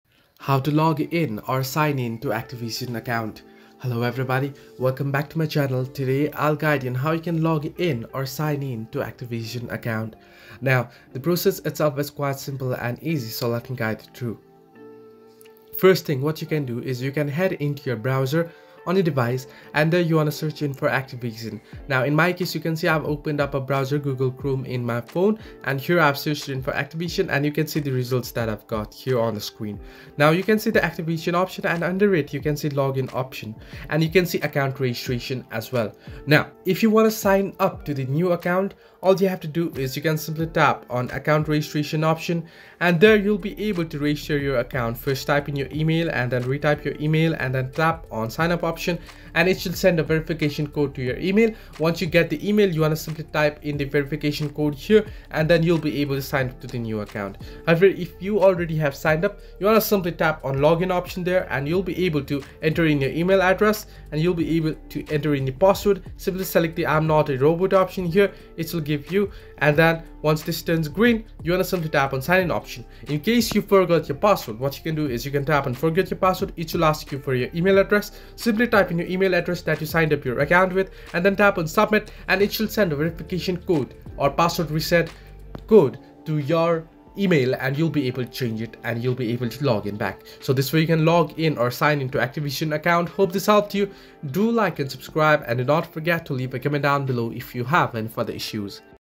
How to Login Activision Id - Sign In Activision Account
In this tutorial video, I will simply show you how to log in or sign up on activision id. So just make sure to watch this video till the end.

~ Video Chapters:
0:00 Introduction
0:18 Log In / Sign In Guide
1:30 Outro